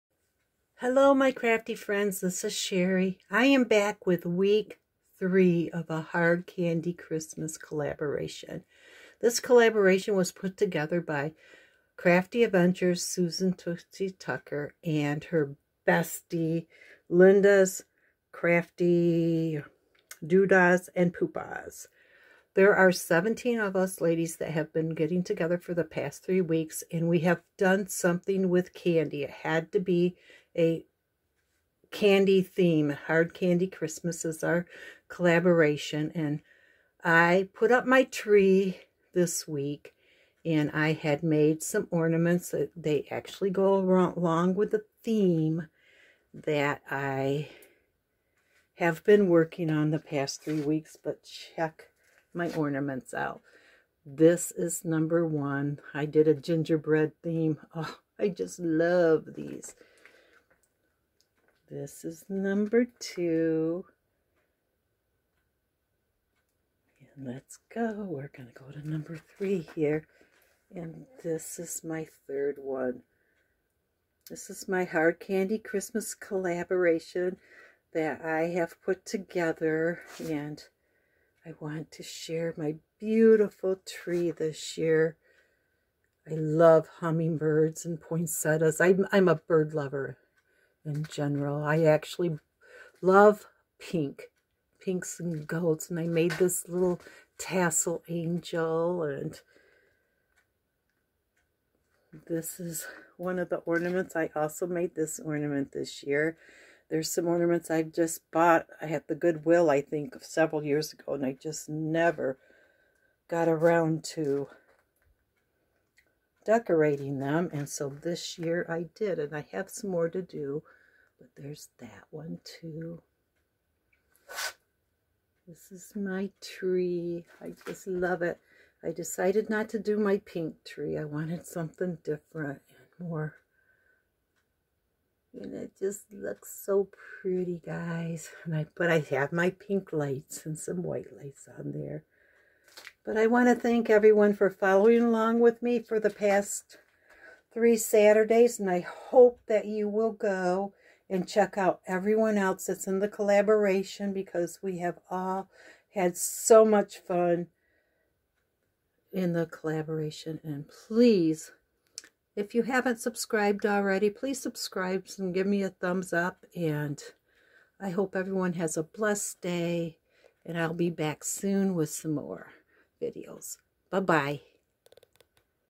ahardcandychristmascollab Week 3
This video is not meant for anyone under the age of 18.
Welcome to week 3 our final week of ahardcandychristmascollab. I hope you have enjoyed the beautiful projects everyone has made. I want to thank everyone that has followed along over the past 3 weeks.
Welcome to the ahardcandychristmascollab. This is a three week Collab with candy as the theme. Follow along and see everyone’s amazing craftiness at work.